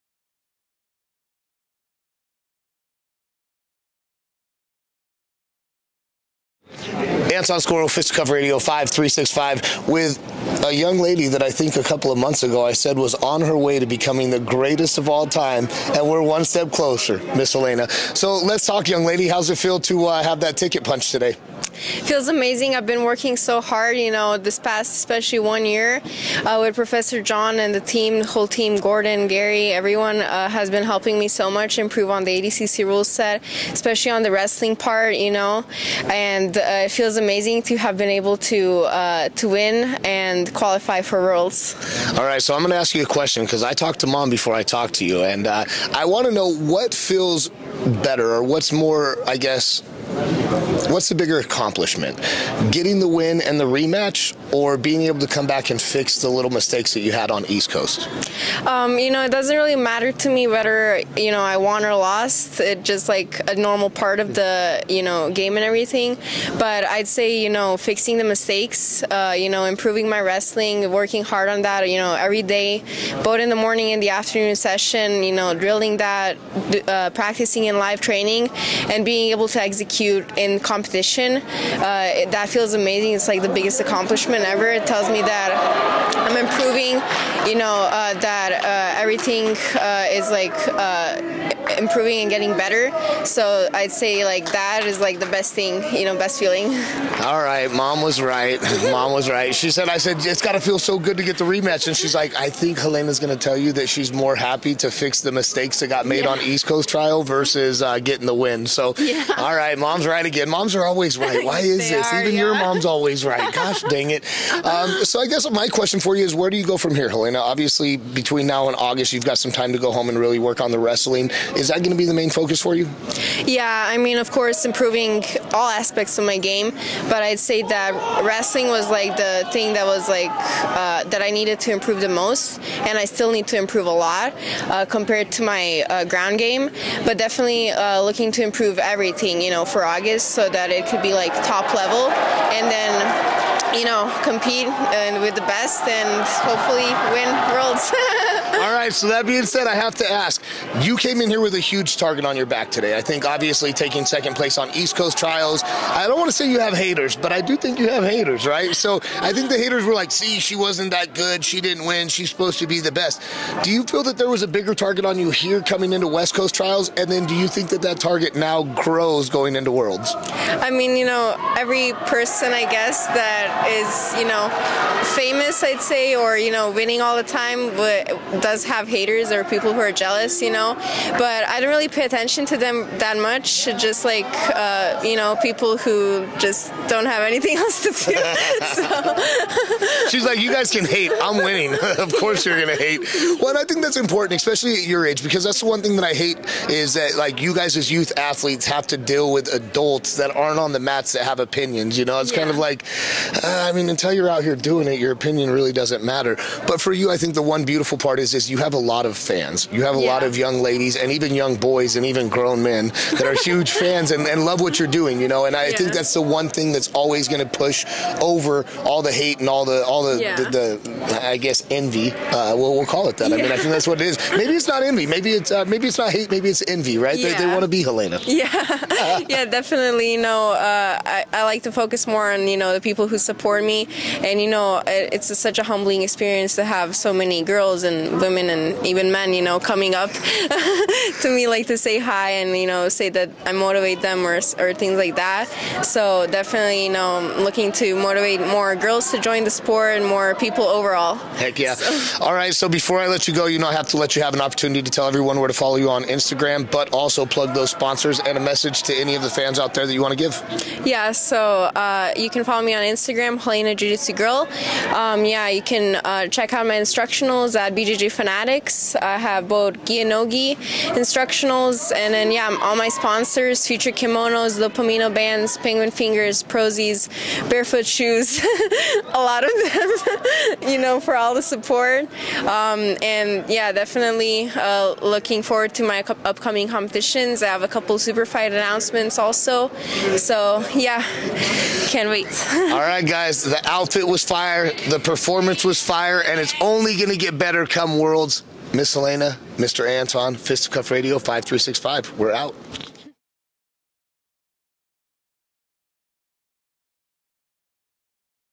Helena Craver Talks ADCC West Coast Trials, ADCC Worlds, & More
Helena Craver gave Anton Skoro & The Road To ADCC Crew some time after her impressive performance at the ADCC West Coast Trials. We talked about her punching her ticket to ADCC Worlds, and fixing the little things between East Coast and West Coast. Helena talked about the target on her back, and how she truly doesn't pay attention to the noise and how her focus is on winning. Helena says the one thing she loves are the fans and the people who support her, which is what truly matters. We hope you enjoy the interview with one of the brightest stars in the sport, and all of our coverage on the Road To ADCC.